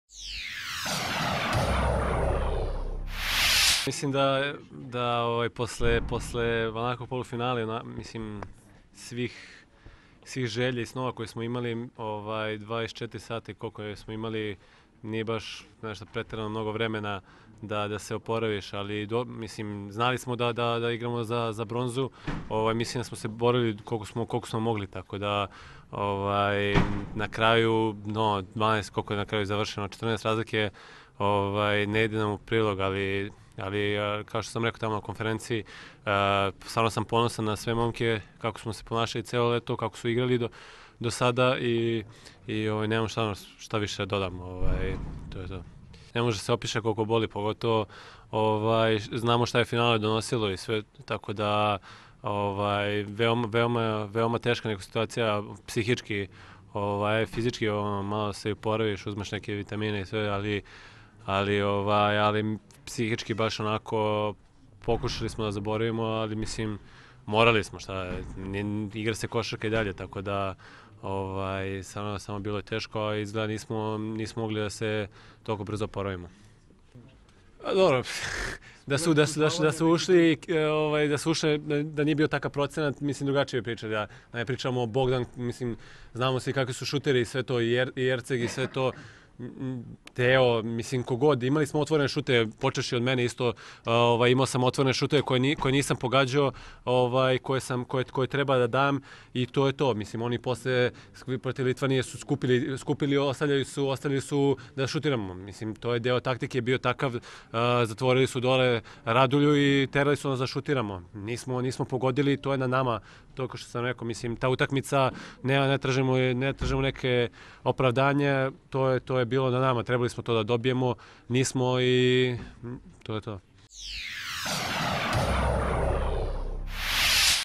STEFAN MARKOVIĆ POSLE PORAZA OD FRANCUZA: Boli, ne može da se opiše koliko boli
Pratite nas i na Viberu ili Vajberu, kako se kome više sviđa

LIL - Košarkaški reprezentativac Srbije Stefan Marković rekao je da je ponosan na ono što je njegova ekipa pokazala na Evropskom prvenstvu i istakao da ga porazi u polufinalu i utakmici za bronzanu medalju "mnogo bole".

"Posle onakvog polufinala i svih želja i snova koje smo imali, 24 sata nije mnogo vremena da se oporavimo. Znali smo da igramo za bronzu, borili smo koliko smo mogli. Stvarno sam ponosan na sve momke, kako su se ponašali celo leto, kako su igrali do sada", rekao je Marković u Lilu.

Košarkaška reprezentacija Srbije je pre dva dana izgubila u polufinalu od selekcije Litvanije, a danas nije uspela da osvoji bronzanu medalju. U utakmici za treće mesto slavila je selekcija Francuske sa 81:68.

"Boli, ne može da se opiše koliko boli. Znamo šta je finale donosilo tako da je psihički bila veoma teška situacija. Pokušali smo da to zaboravimo, ali izgleda da nismo mogli toliko brzo da se oporavimo", naveo je Marković.

Srbija je na šampionat Evrope otišla kao vicešampion sveta, pa su je svi protivnici, prema mišljenju Markovića, ove godine mnogo bolje poznavali.

"Ovaj posao donosi pritisak, bilo bi lako da nema pritiska. Pritisak je tu da bi bio bolji. Prošle godine nas niko ni u Srbiji, a kamoli u svetu poznavao, išli smo borili smo se... Ove godine je bilo drugačije. Sve ekipe koje smo dobili prošle godine su sada znale ko smo i šta smo, znali su odakle preti najveća opasnost", rekao je Marković.

"Bili smo na pravom putu, do polufinala je sve bilo dobro, ali taj i današnji poraz su malo pokvarili utisak", dodao je on.

Srbija je osvajanjem četvrtog mesta na Evropskom prvenstvu izborila učešće u kvalifikacijama za plasman na Olimpijske igre u Rio de Žaneiru.

"Ostaje nam to kao utešna nagrada. Spremićemo se, imamo celu sezonu da se oporavimo od ovoga i da se borimo za to što svi sanjamo", rekao je Marković.